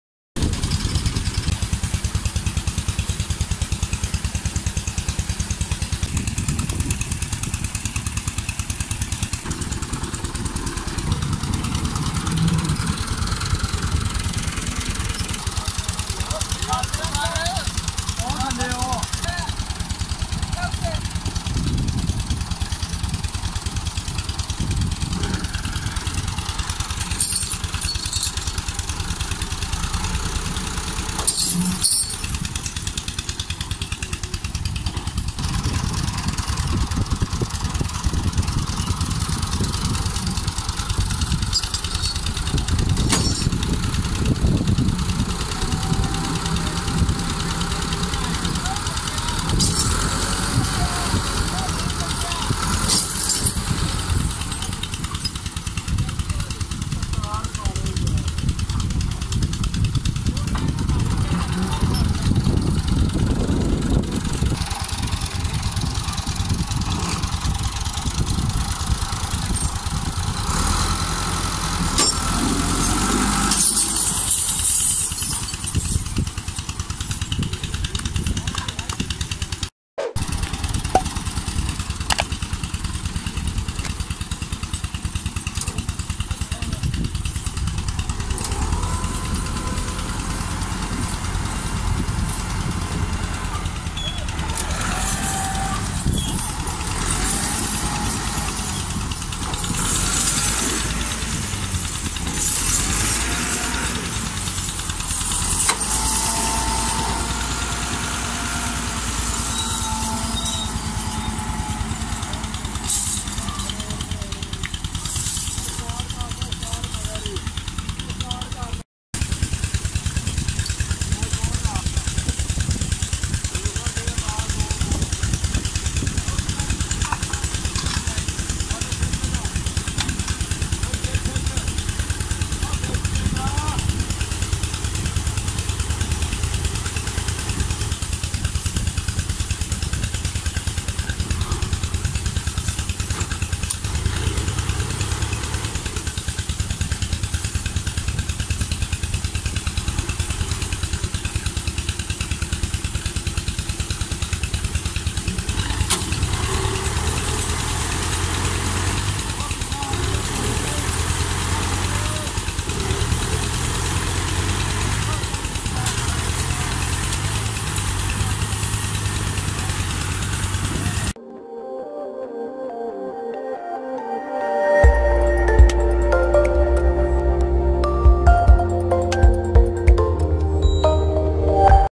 Ghazi 480 Tractor Stuck Badly | Stranded NEWHOLLAND Tractor | Pk Business Information
Assalam_O_Alaikum friends, how are you, I hope you all are well
In this video we will see how to pull a stranded tractor, tractor are trying to pull out a tractor and finally manage to pull out the tractor

In this channel you'll see
1: Videos about all company Tractors reviews and their performances with different type of implements
3: Videos about business ideas

Owner of Channel:
Abubakar Ghumman